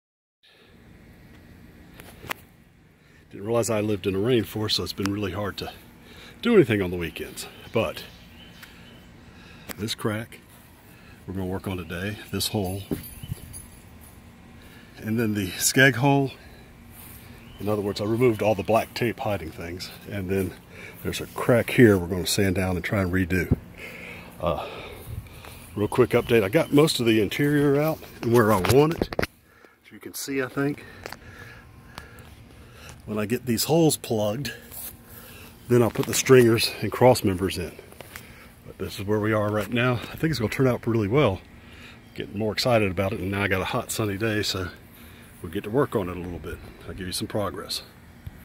Sunfish to skiff
Fiberglassing the holes and some cracks